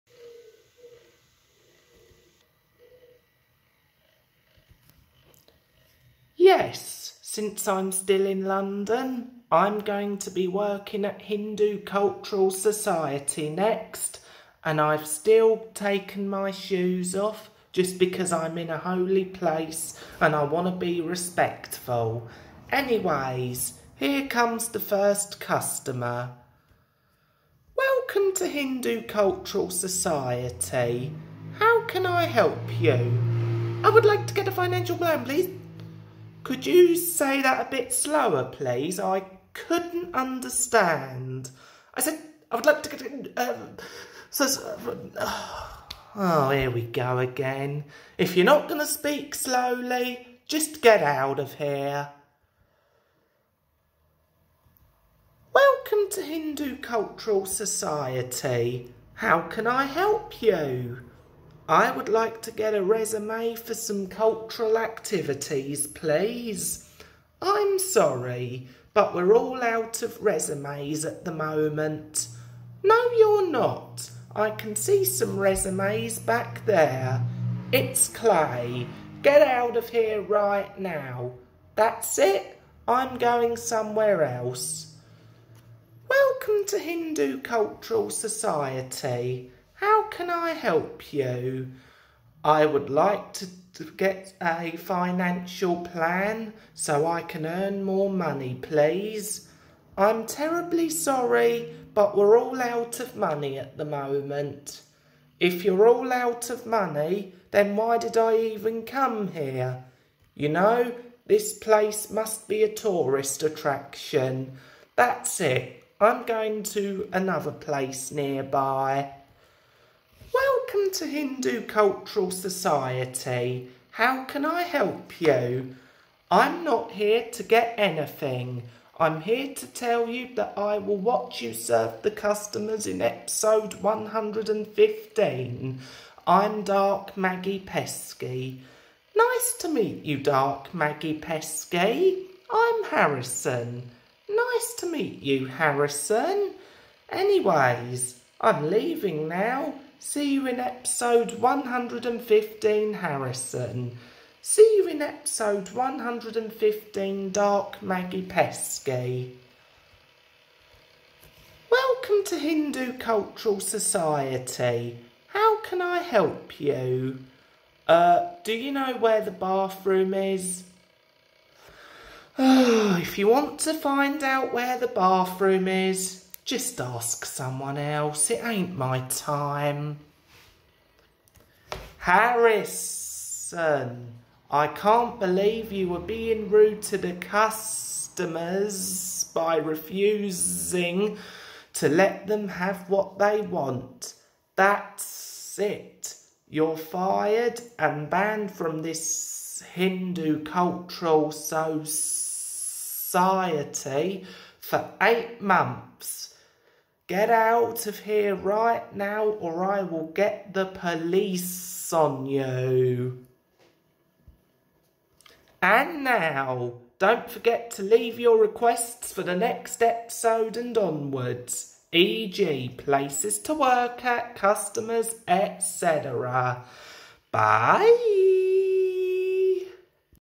Harrison For Hire #112: Hindu Cultural Society
Welcome to the 112th episode of Harrison For Hire! After getting fired at Vishawa Hindu Mandir, I decide to go and get a job at Hindu Cultural Society!

NOTE: The next episode will feature 6 Dark Fire Bro Clones!